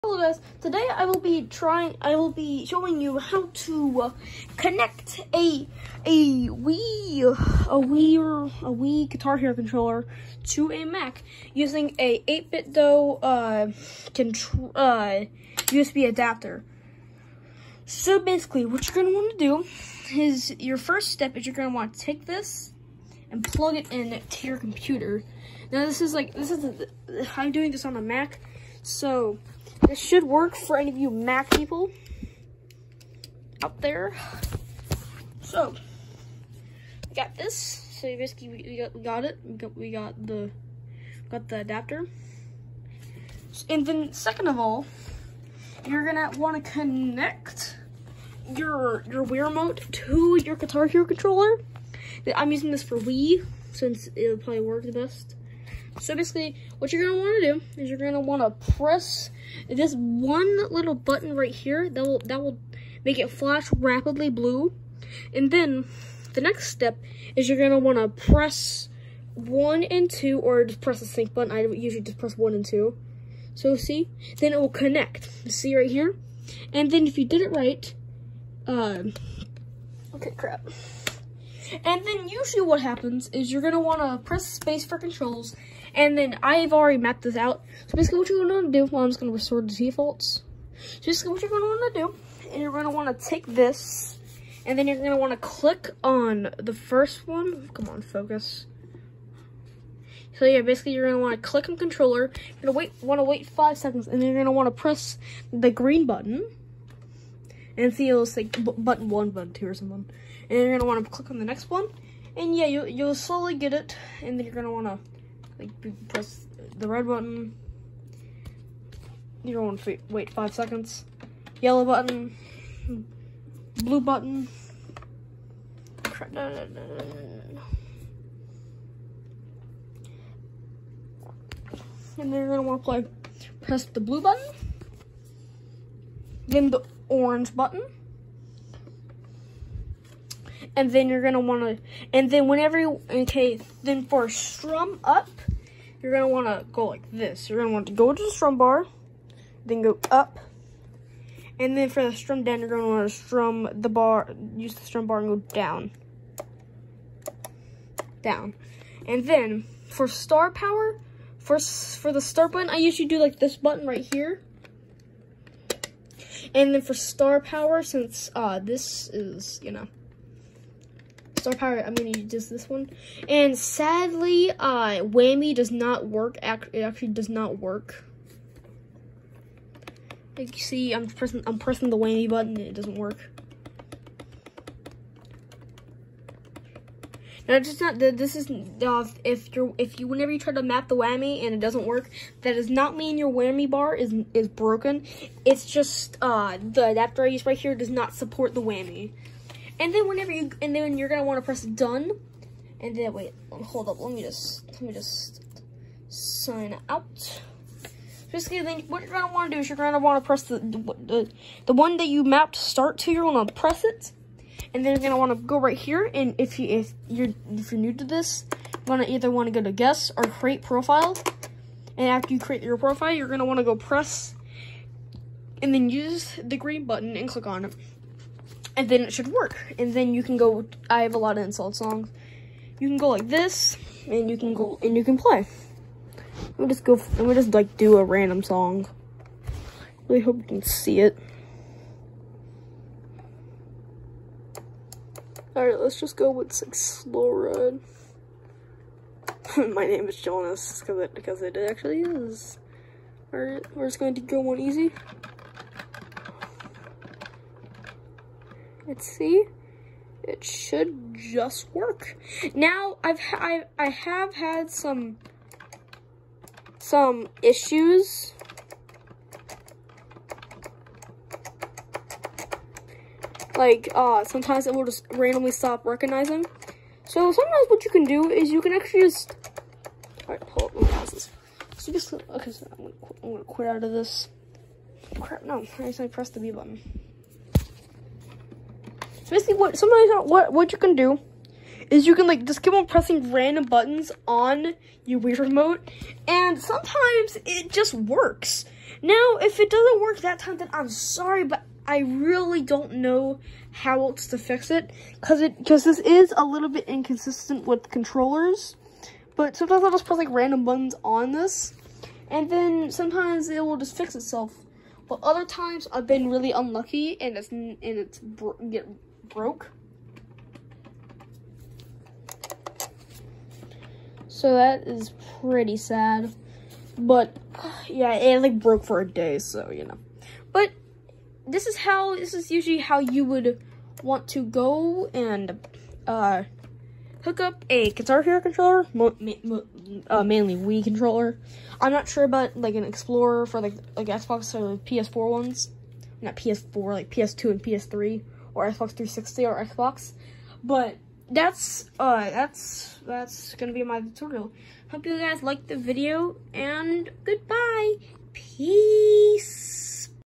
How To Connect A Guitar Hero Controller to Mac
Sry not what I usually do for a vid but I could never find a tutorial that worked on the newest version of Mac so I decided to make one to help mac clone hero players

ADAPTER I USED